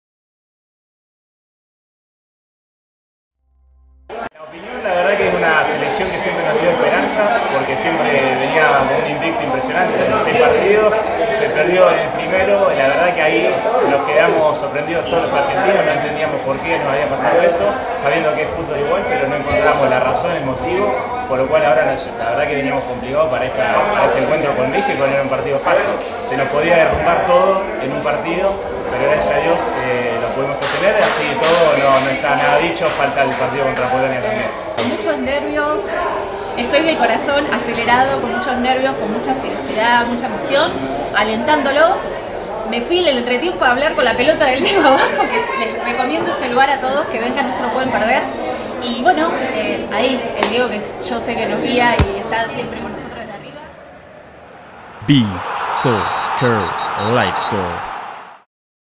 De la tensión a la euforia bajo la atenta mirada del '10'
El ambiente en Buenos Aires estaba enrarecido este sábado, en el que el silencio y la tensión se apoderaban de la gente, nadie hablaba o se miraba a la cara directamente; con camisetas y portaestandartes de color celeste y blanco, todos los pensamientos estaban en el Mundial de Qatar.